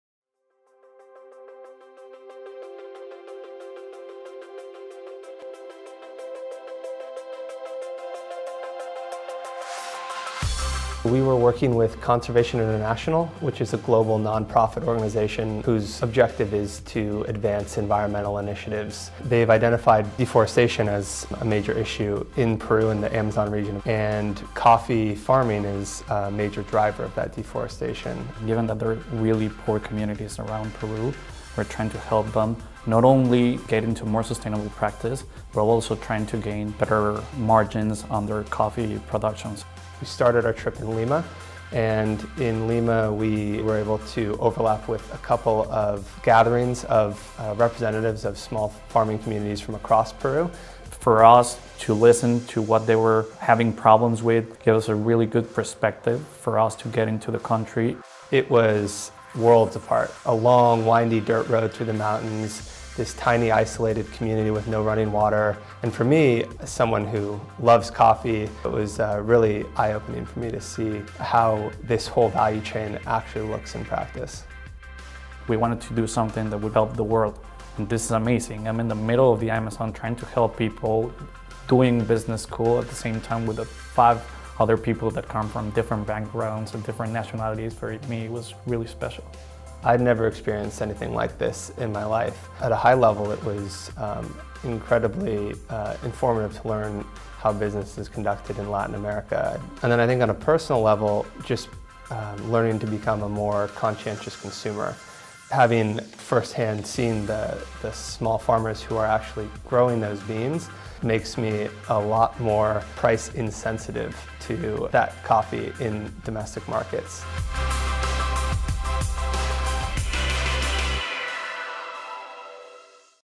2018 Developing an Environmentally Sustainable Value Chain for Coffee Production in San Martin, Peru
Conservation International (CI) is a global nonprofit organization that helps to build a healthier, more prosperous and productive planet. Over the last 10 years, CI has established a growing presence in Peru and identified coffee farming as a key driver of deforestation in the country, especially in the San Martín region. Building on the work of past projects, CI engaged the AMR team to support the continued development of an environmentally sustainable value chain for coffee production in the San Martín region. The goal of the project was to identify the key enabling conditions and make recommendations for scaling current efforts to improve sustainability by identifying optimal linkages in the value chain for intervention. The team researched how best to incentivize local farmers, while considering the social and economic impacts on the small communities.

After several weeks of extensive secondary research, team members traveled to Lima and the San Martín region to conduct their primary research, which was essential to discovering how cultural, economic and social systems work within the small local communities, and also afforded exposure to best practices and challenges in engaging with small farmers. The students conducted interviews with coffee industry players and subject matter experts to learn about social responsibility and sustainability initiatives, key considerations for sourcing, and consumer trends relating to coffee consumption and the importance of a sustainable value chain. They also met with smallholder farmers, local community representatives and leaders of cooperatives to identify how best to engage with member farmers, foster loyalty and ensure quality in the collective final product and better understand why local communities were not engaging in more sustainable farming practices. In Lima, they attended a monthly meeting with the National Coffee Board and later flew to Tarapoto to spend a day immersed in the activities of the successful Oro Verde cooperative based in Llamas.

The team developed three primary recommendations to support the development of a more sustainable value chain for coffee production in the San Martín region: pursue value-added activities, promote local certification and support expansion of cooperatives. The students found that small farmer financial security needs to be addressed and improved before they can get involved in conservation initiatives. If the San Martín farmers establish cooperatives to collectively negotiate for better prices, it could increase their income significantly. Cooperatives can also attract the business of specialty coffee companies, which can substantially increase small farmer income. Consumers are also increasingly choosing specialty coffee, which drives premiums in coffee market prices.